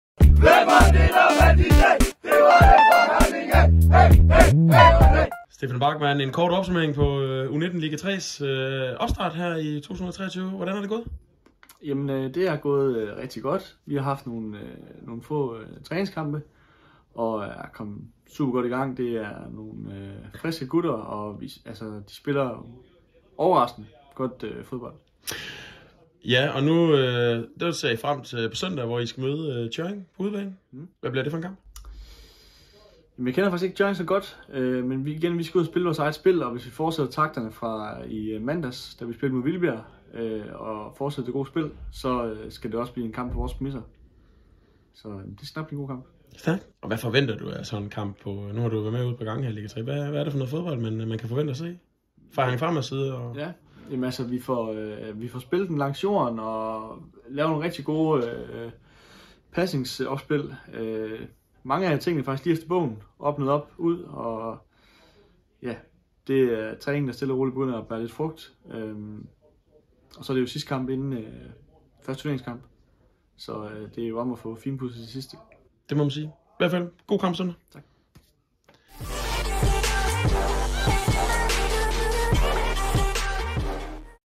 U19 Lig-3 møder Tjørring IF
U19 Liga-3 har haft en rigtig god opstart og søndag kl 16:00 spillet holdet sidste træningskamp inden turneringen går i gang. Der sker når holdet tager til Tjørring ! Lidt rivalisering er altid godt, STOREBROR / lillebror imellem 😉

Vi talte med Stefan Bachmann inden opgøret ! se med her:

FORZA FREMAD 💙